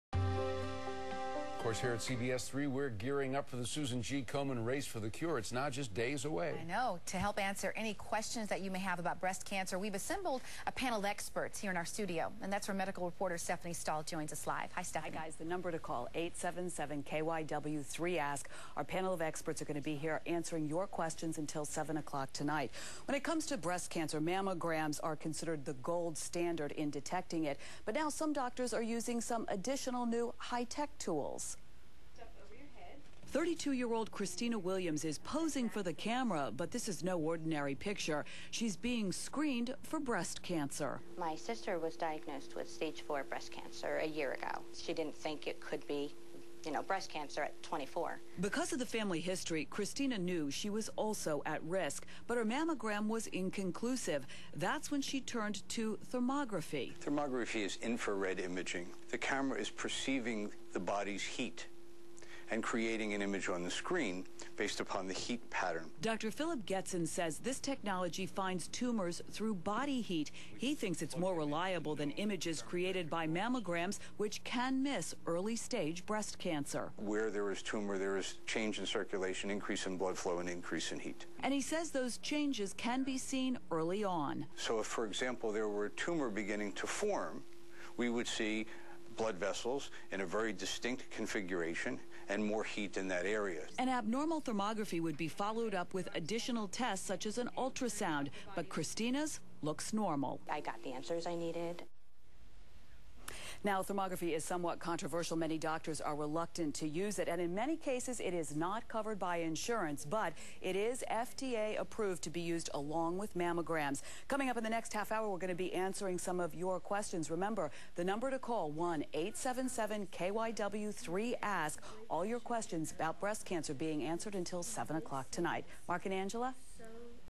Dr. Getson - Thermography Interview KYW Channel 3 TV News
Interview with Dr. Philip Getson about thermographic imaging and the early detection of breast cancer.